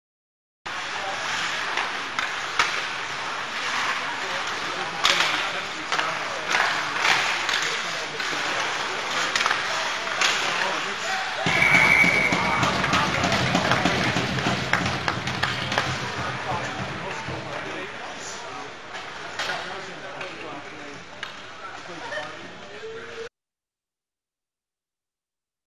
Luc Fricchione Backhand goal
U16 USPHL Luc Fricchione jump in for goal